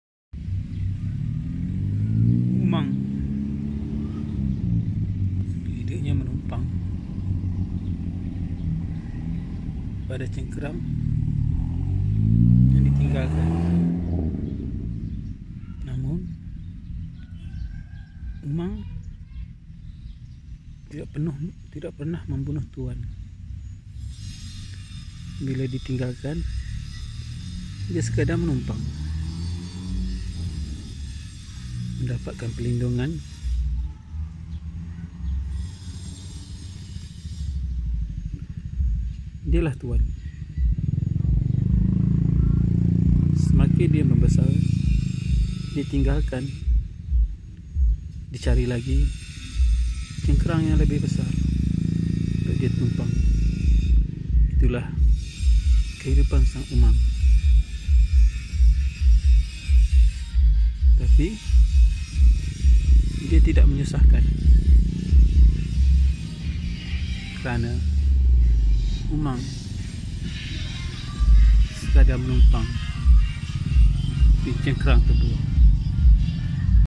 Umang satu keajiban
Umang umang biasanya ditermui di air rupanya ada juga umang umang yang tinggal di darat.